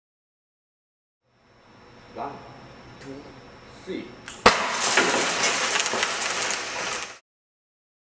Escort Claymore
Made of a high density injection molded polypropylene plastic (think fishing tackle box) and nearly 100% accurately resembles the real M18a1 mine. Video supplied by Paul, (ClassicAirsoftGuns)